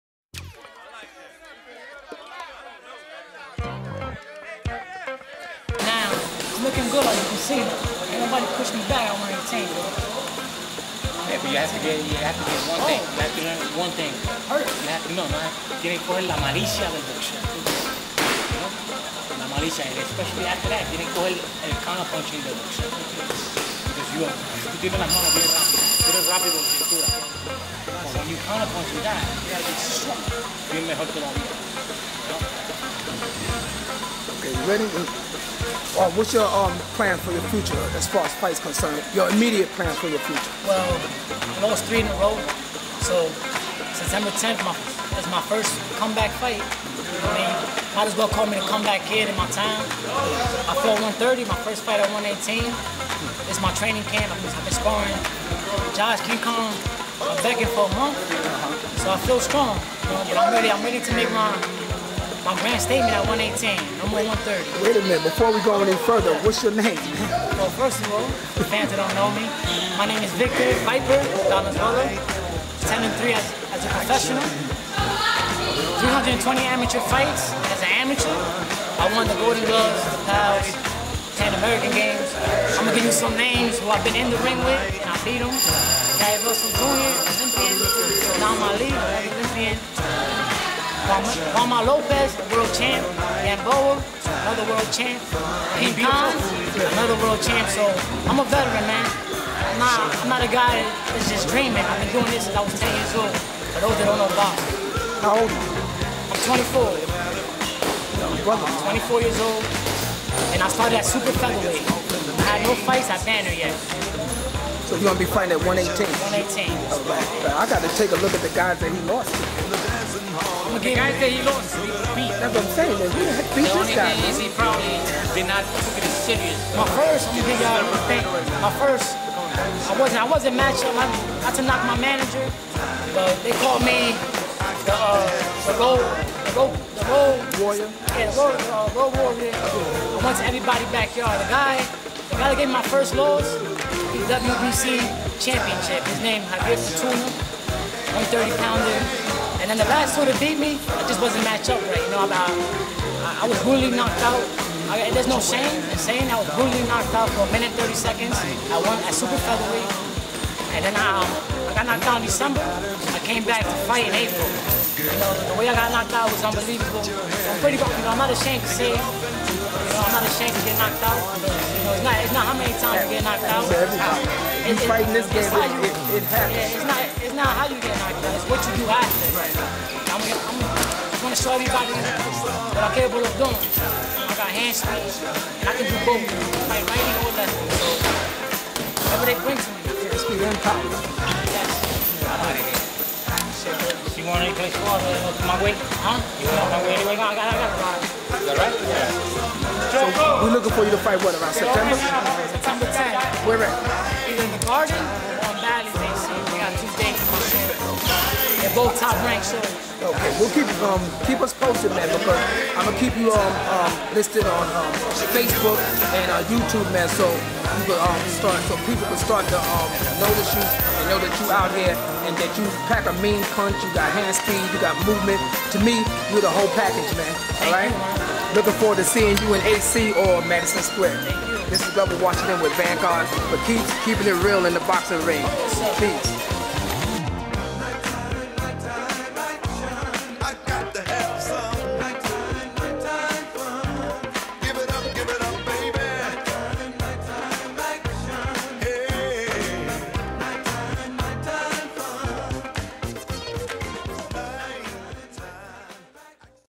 In This Corner: The Viper
Young Pro boxer is introduced to the public. This young man has all the gifts to go far in the sport of boxing. Keep your eyes on him.